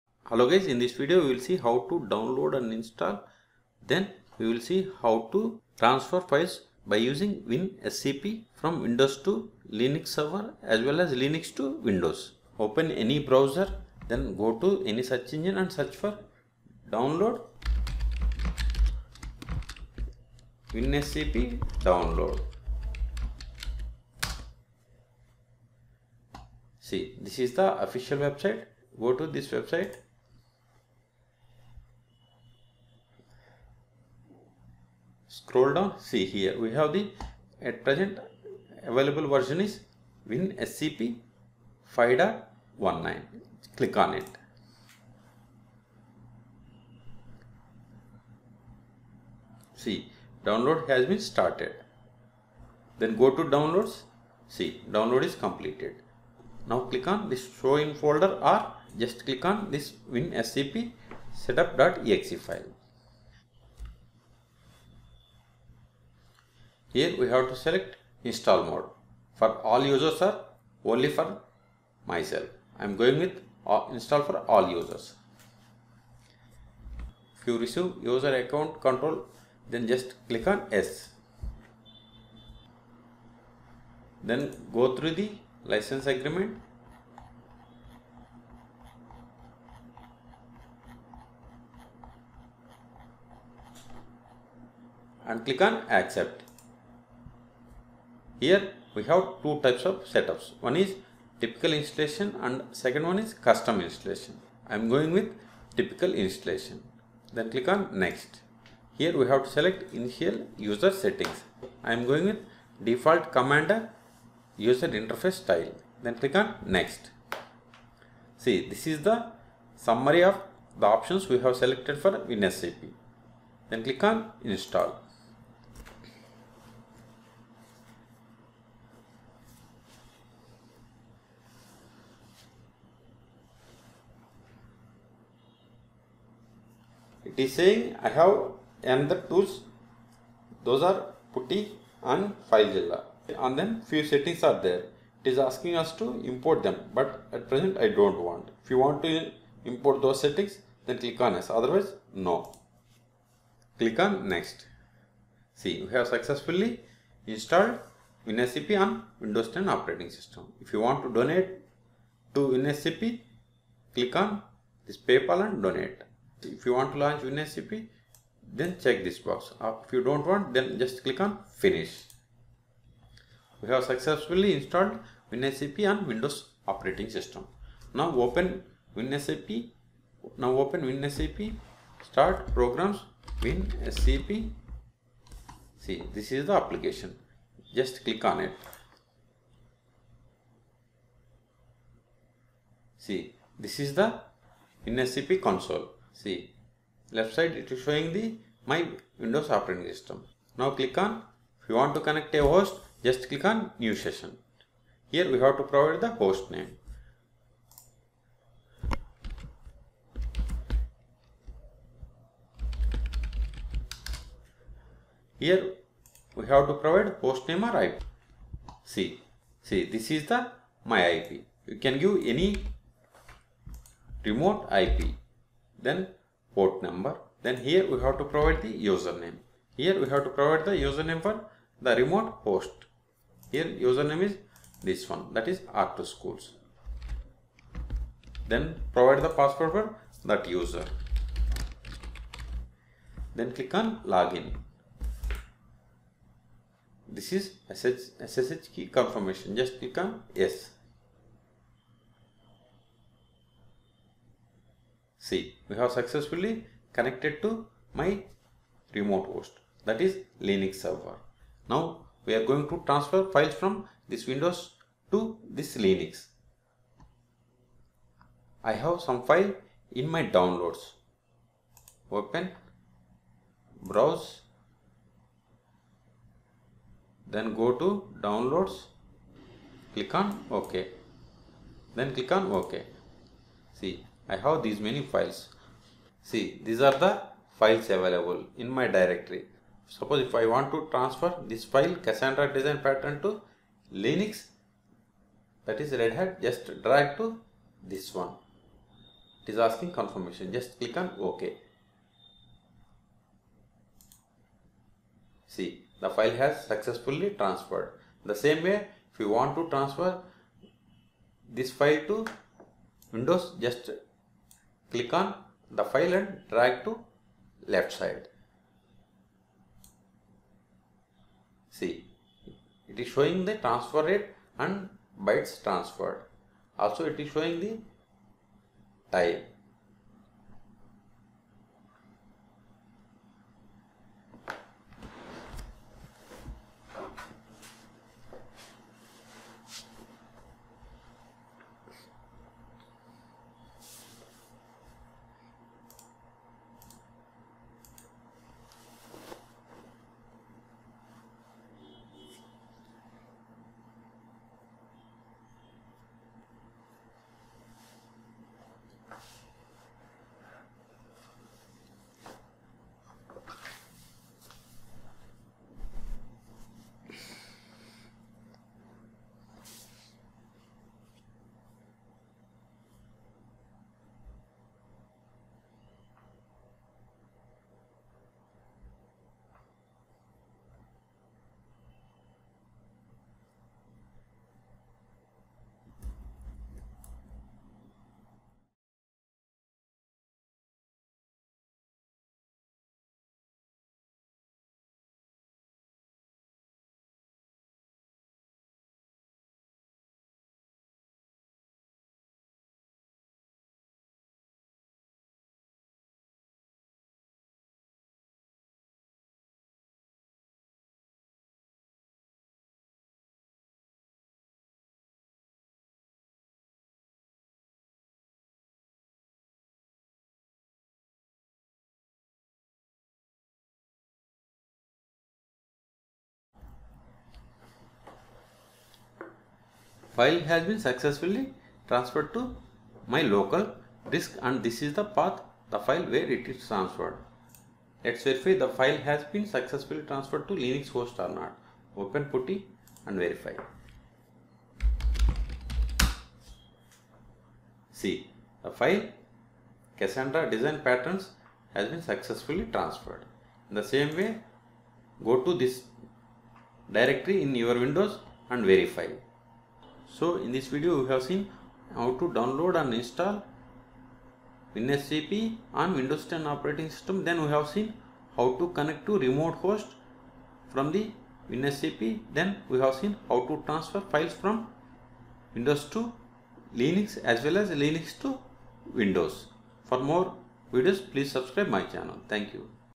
How to download install winscp and transfer files between Windows and Linux
In this video, we have shown How to download install winscp and transfer files between Windows and Linux.